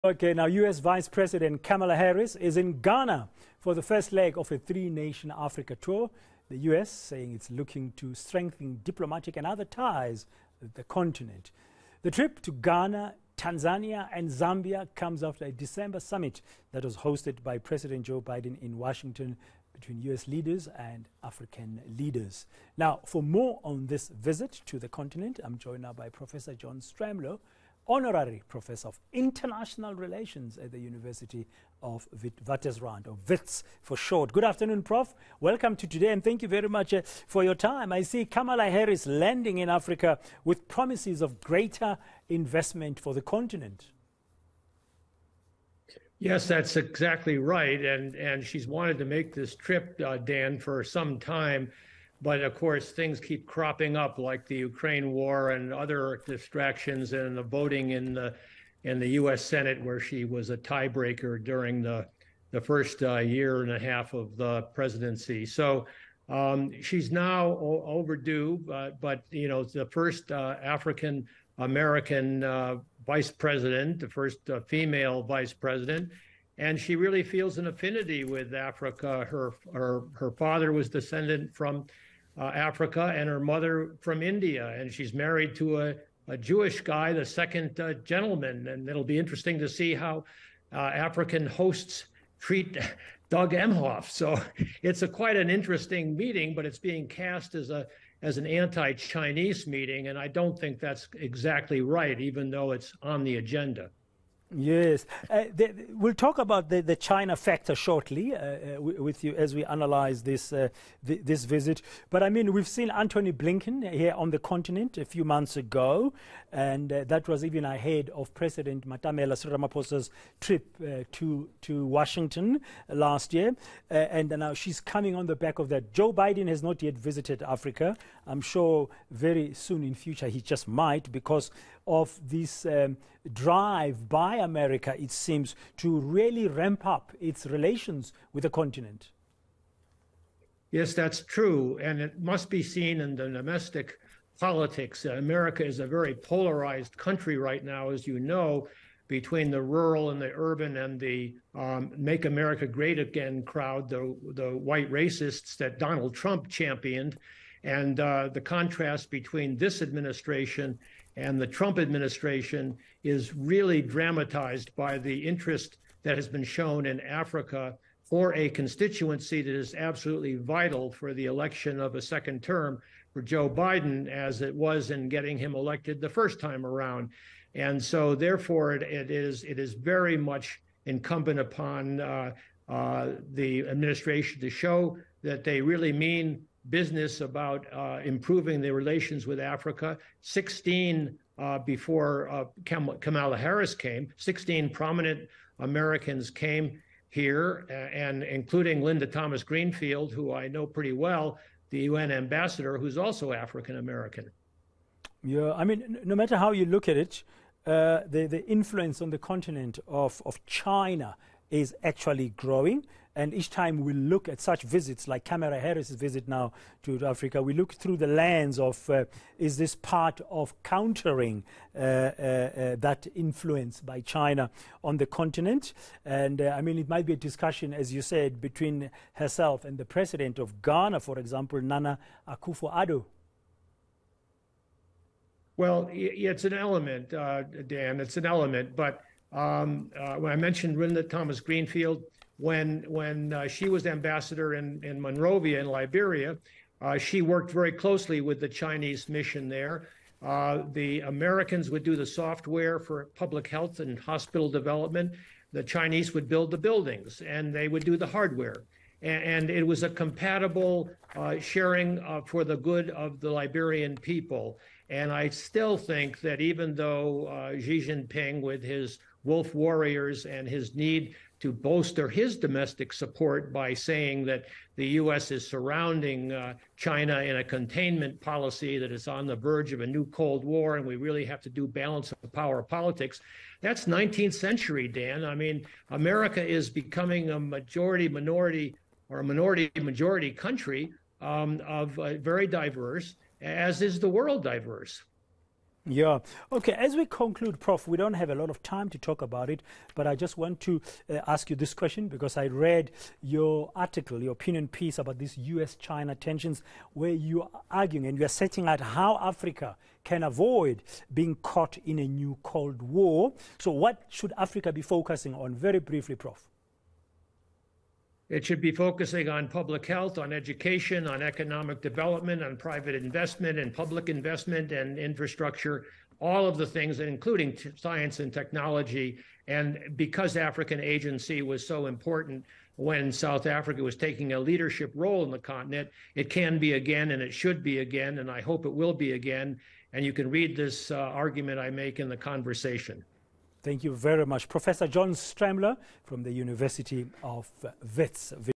US VP kicks off three nation African tour
US Vice President, Kamala Harris, is in Ghana for the first leg of her three-nation Africa tour. The US is looking to strengthen diplomatic ties on the continent. For more, eNCA speaks to Professor John Stremlau, the Honorary Professor of International Relations at the University of Witwatersrand.